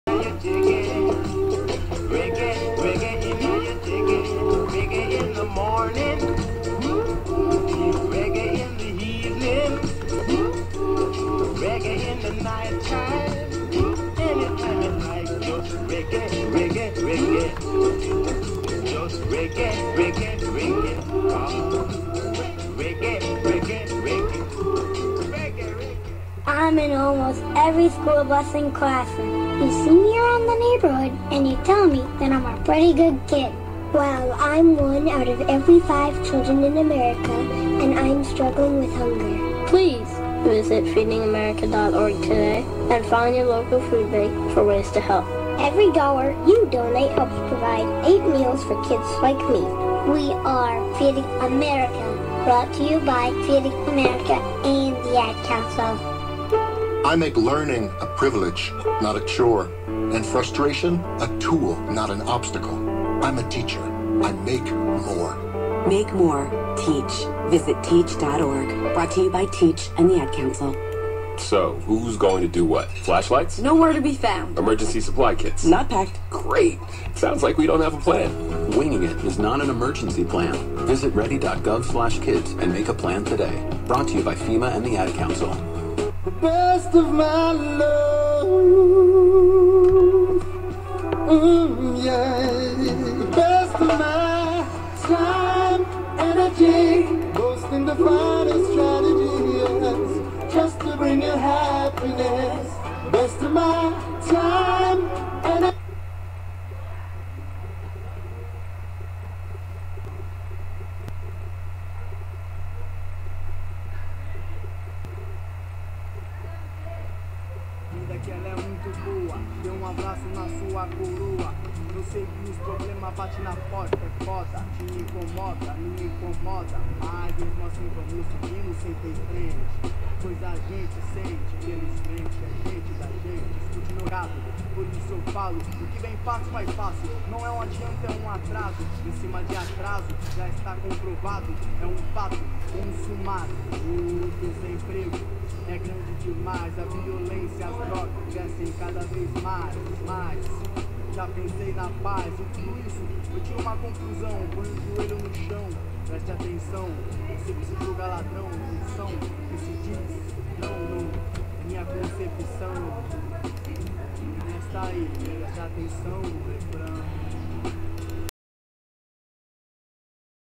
Como Escutar Rádio pelo Windows Media Player [2017]
1º voce precisa do ip da radio , aqui abaixo tem alguns ex:

  2º copie o endereço ip

  3º abra o Midia Player e use o atalho , ctrl U,

  4º cole o ip , precione OK

   pronto :)